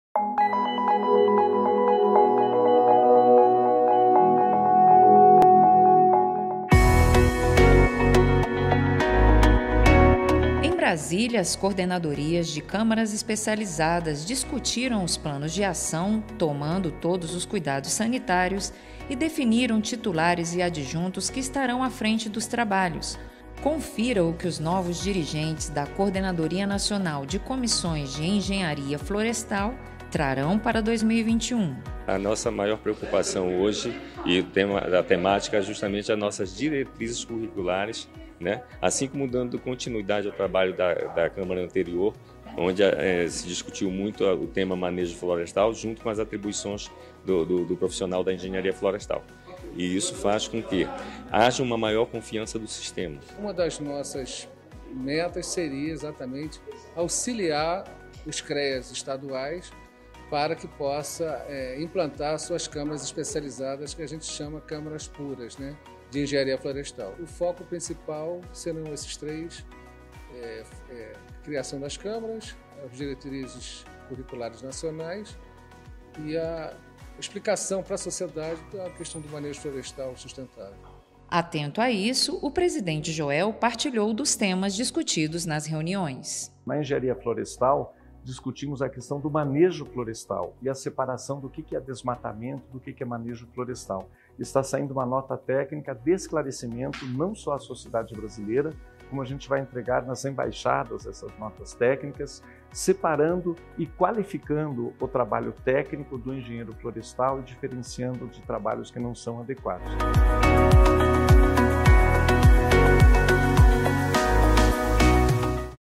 Entrevista com os Coordenadores da Coordenadoria de Câmaras Especializadas de Engenharia Florestal
Entrevista da TV Confea com os Coordenadores da Coordenadoria de Câmaras Especializadas de Engenharia Florestal.